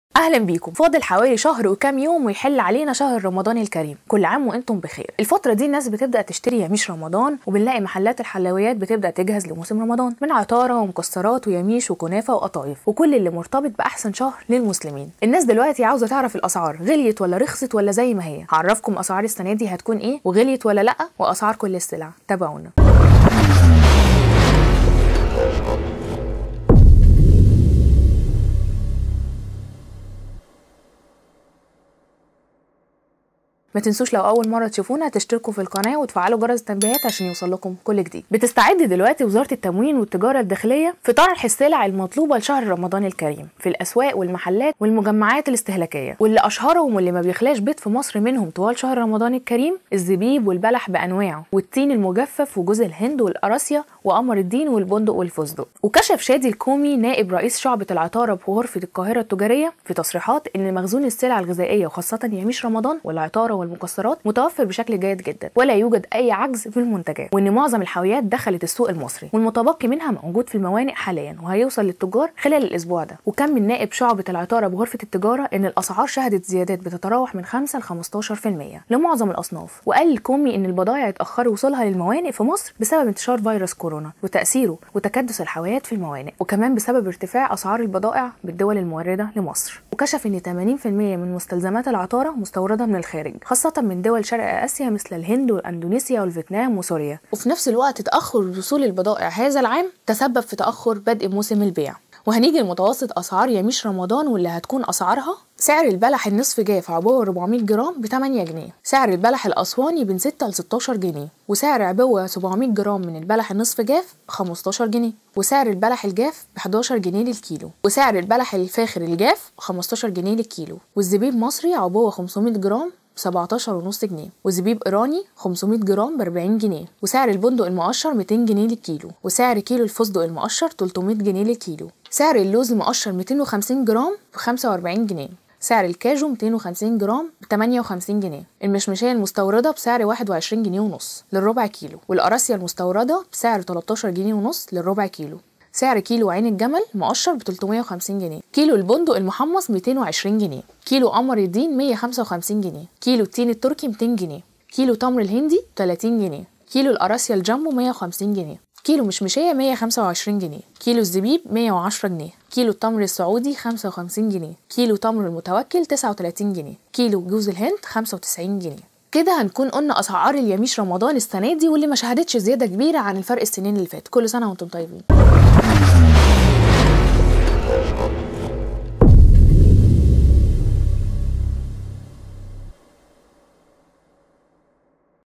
ارتفاع أسعار ياميش رمضان ٢٠٢١.. بالتفاصيل ننشر الأسعار وأسباب تأخر البيع في المحلات
فاضل حوالي شهر وكام يوم ويحل علينا شهر رمضان الكريم، كل عام وانتم بخير، الفترة دي الناس بتبدء تشتري ياميش رمضان وبنلاقي محلات الحلويات بتبدء تجهز لموسم رمضان من عطارة ومكسرات وياميش وكنافة وقطايف وكل الي مرتبط باحسن شهر للمسلمين في السنة.. الناس دلوقتي عاوز تعرف الاسعار غليت ولا رخصت ولا زي ماهي هعرفكم  اسعار السنة دي هتكون ايه وغليت ولا لا واسعار كل السلع تابعونا..